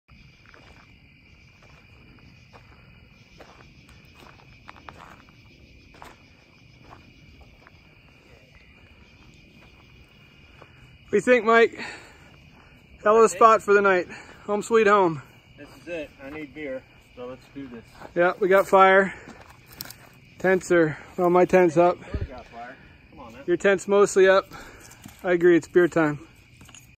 Campsite located. Engage beers.
Overnite camp on the crf450l.  Beautiful spot.